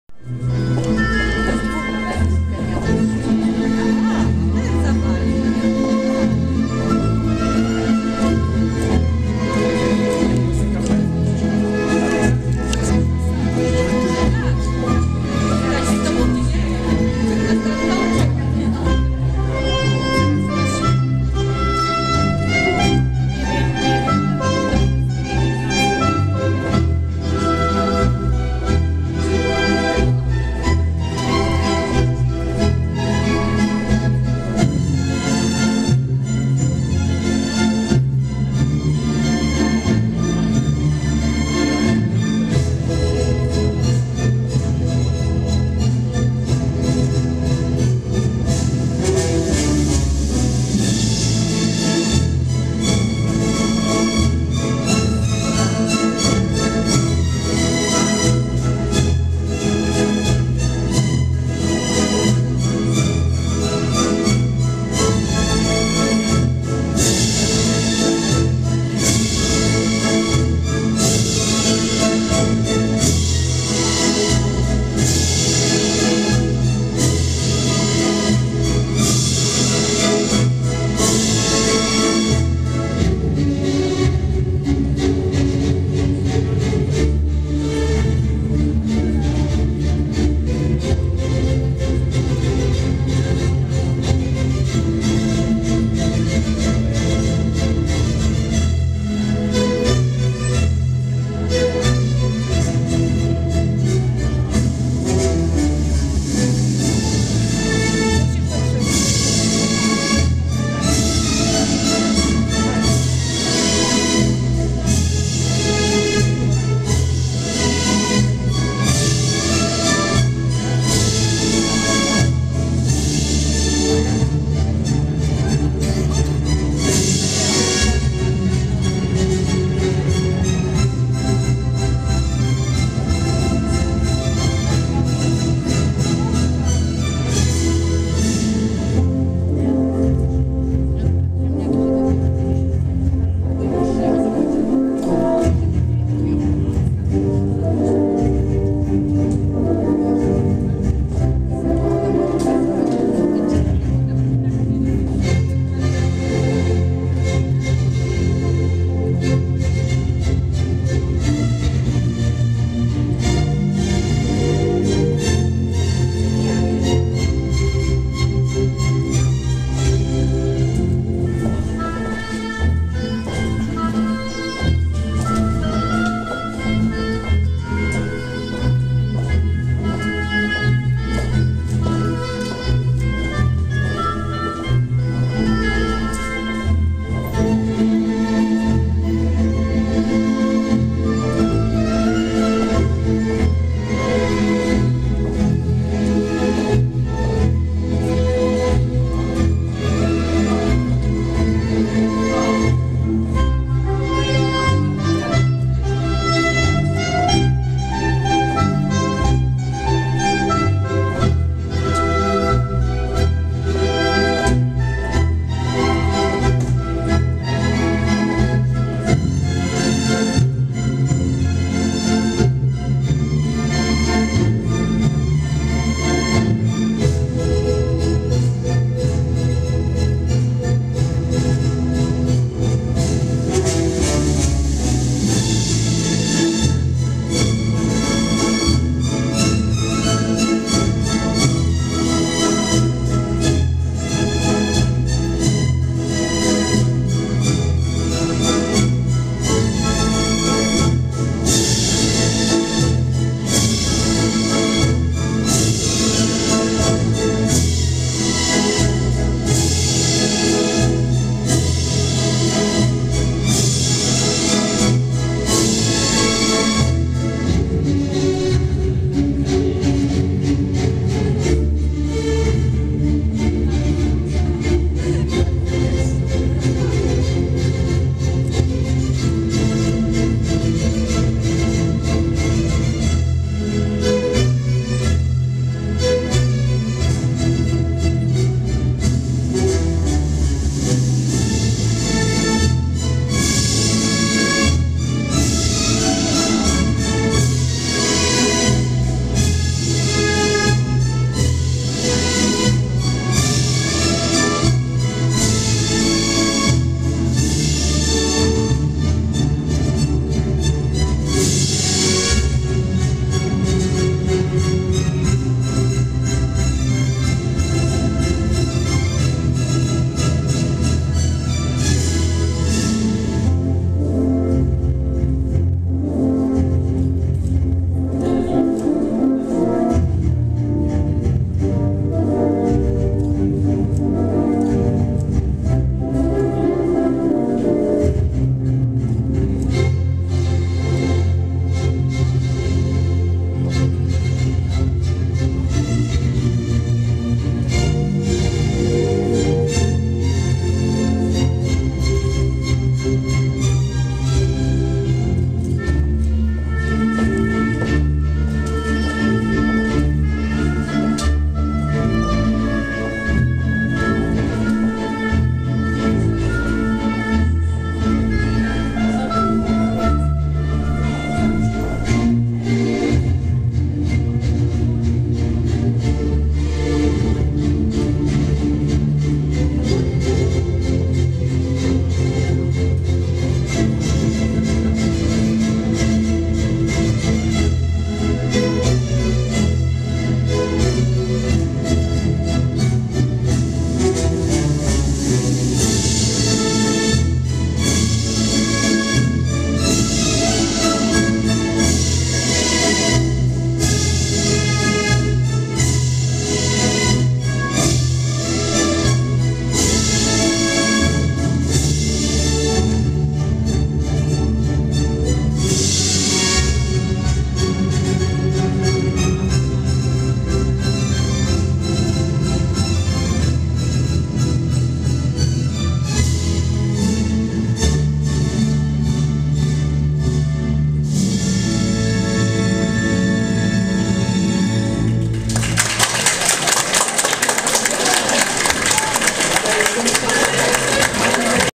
Komers 2015 - III gim
Komers klasy III gim z Zespołu Szkół w Radomyślu nad Sanem. Uczniów przygotowywała pani Danuta Pater.